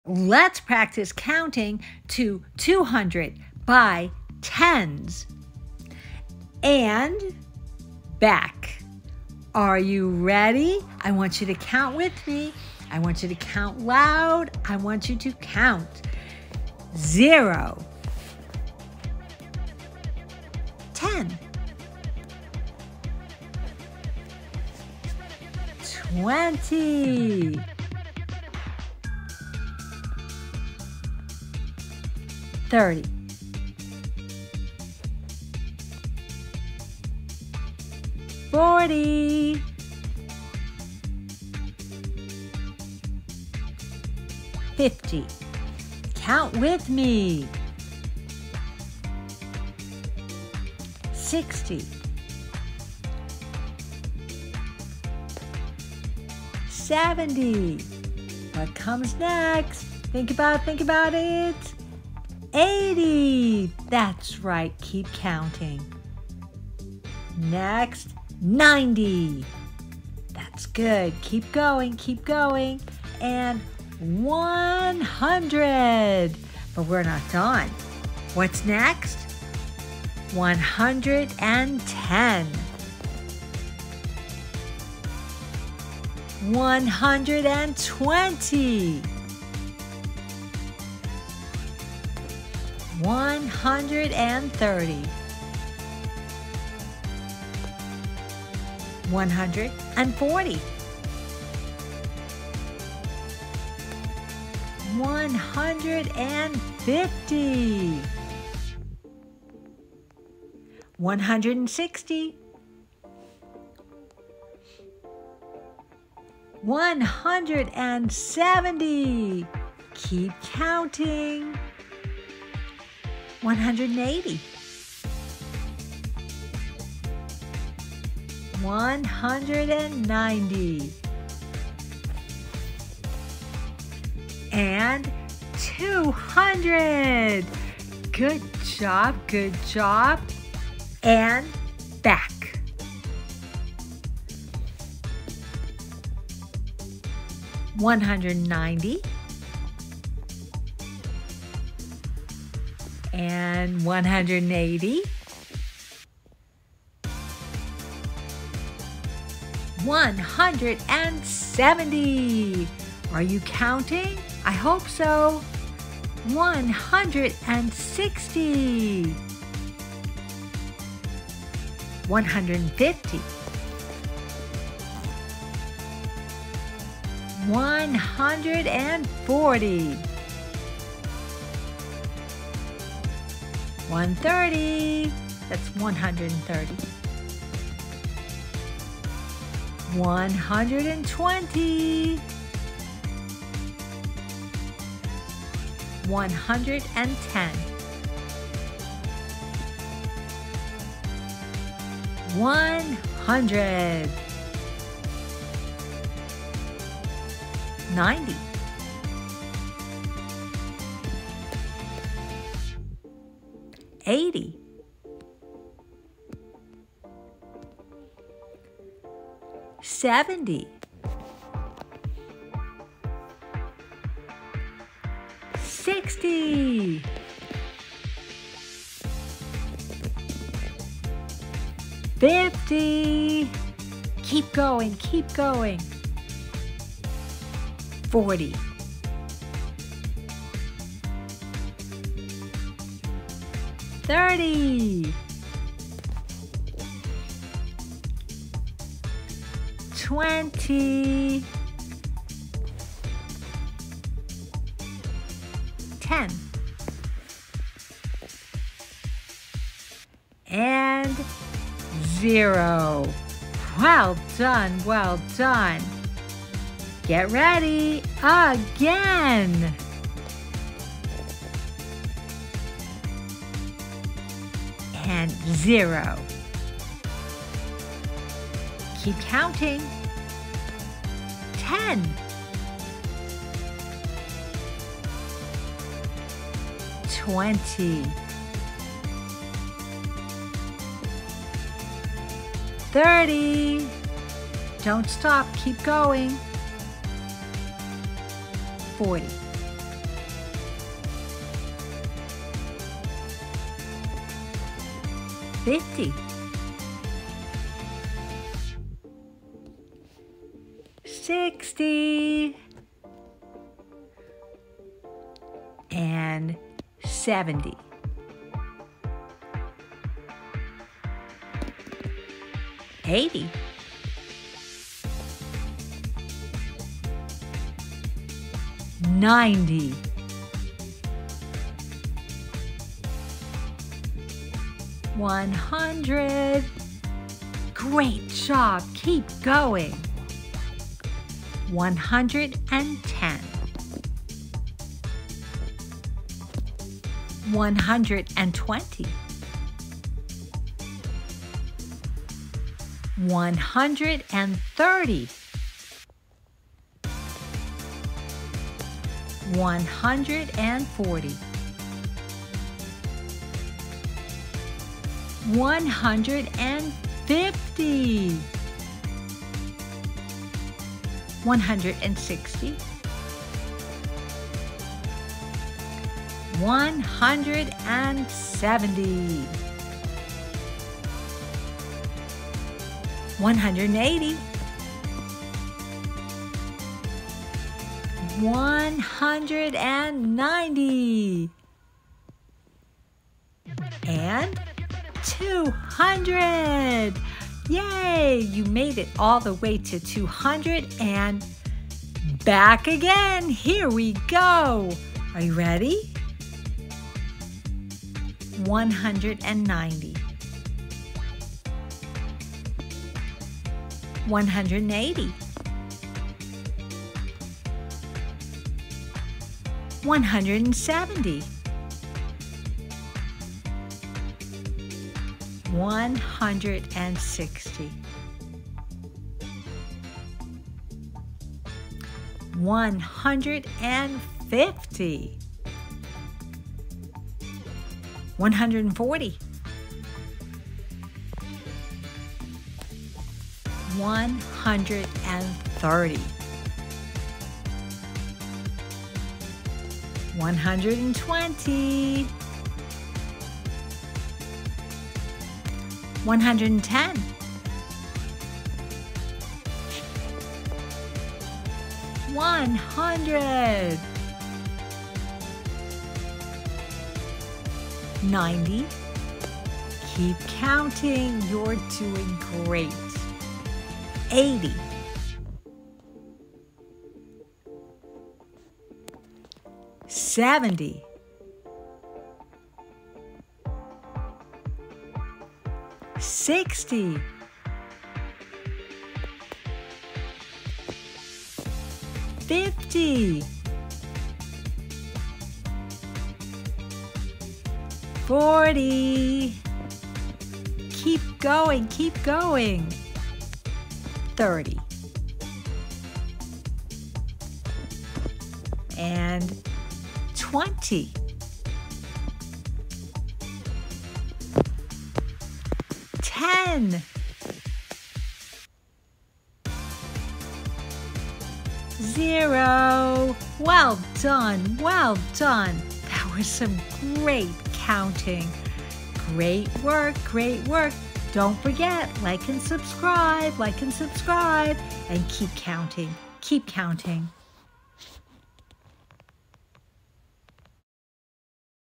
Counting to 200 by 10’s and back (twice)!
Let’s practice counting to 200 by 10’s and back. Two times! Count and have fun with the cool music!